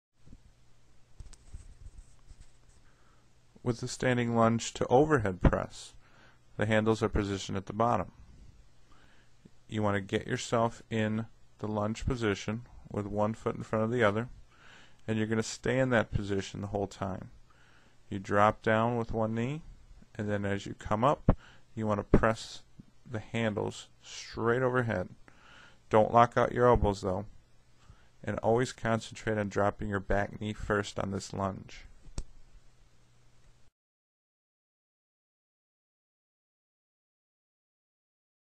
GDCC200 Cable Crossover Exercises - Standing Lunge to Overhead Press
The Body-Solid GDCC200 brings form to function with limitless range of motion and full adjustability in a new, space-saving design.